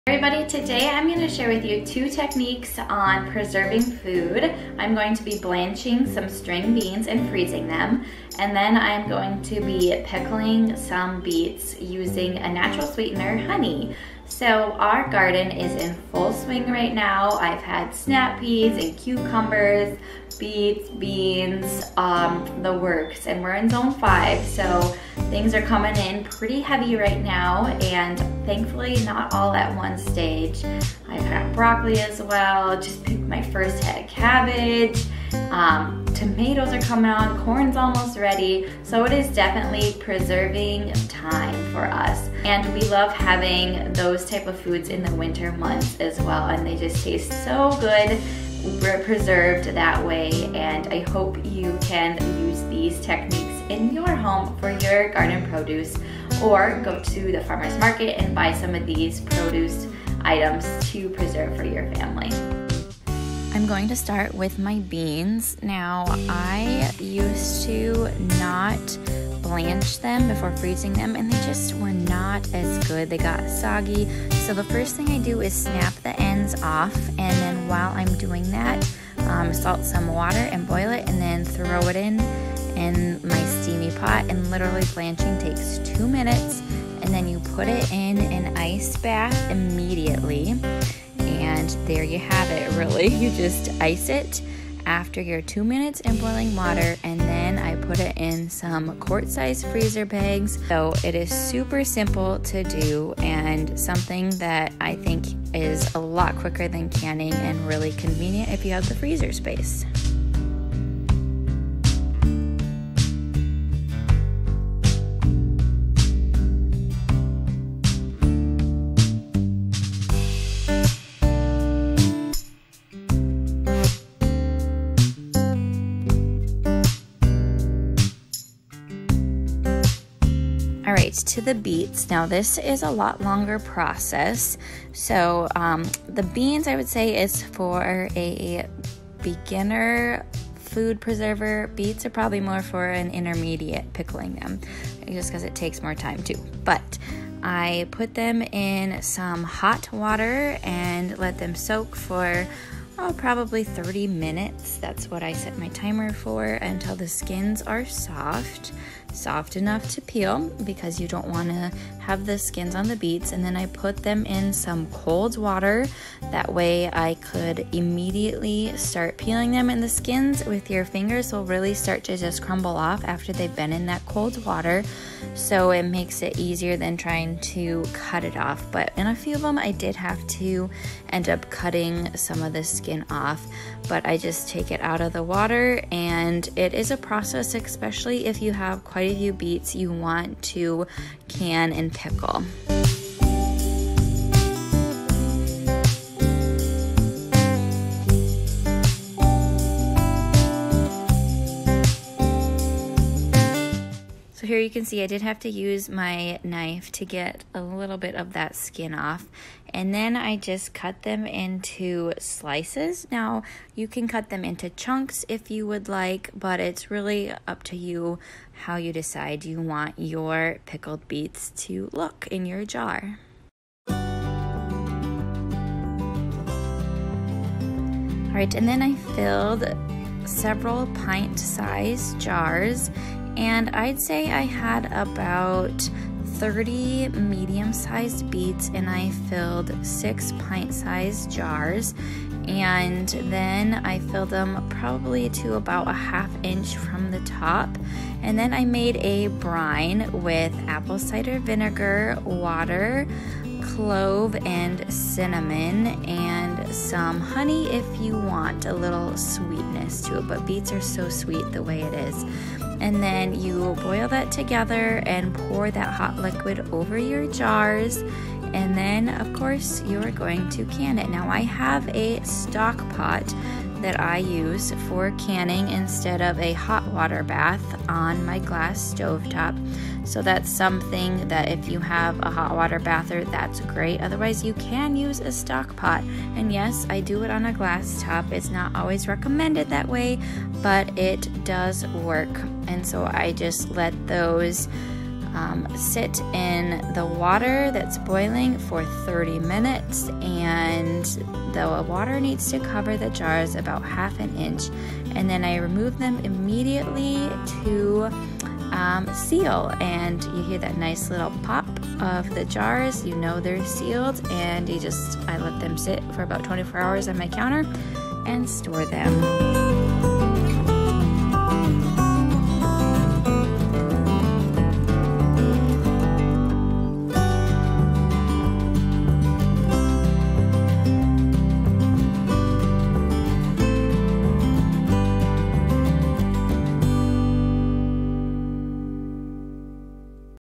Preserving Foods | Honey sweetened Pickled Beets & Blanched green beans for freezing
Hello everyone.  Today I am sharing how to blanch green beans to freeze and how to make honey sweetened pickled beets.  The garden produce is in full swing and what better way to use this produce than to preserve it for winter months.

Pickled Beets:

20-30 med. beets
3 cups apple cider vinegar
3 cups water
6 whole cloves
1/4 cup honey
1 cinnamon stick

6 washed pint size jars, lids & rings

1. Cut tops off beets and wash.
2. Fill pot with water and bring to a boil.  Add beets and boil for about 30 minutes or until tender.
3. Remove from water and place in cold water until easy enough to handle.
4. Remove skin and cut into slices or chunks.  Place into jars.
5. Put the other ingredients into a pot and bring to a boil for about 3 minutes.
6. Pour liquid into jars about 1/2 inch from rim.
7. Place lids and rings on jars and process in a hot water bath or large stock pot for 30 minutes.
8. Remove and listen for the sealing sound.

200 East Park Ave.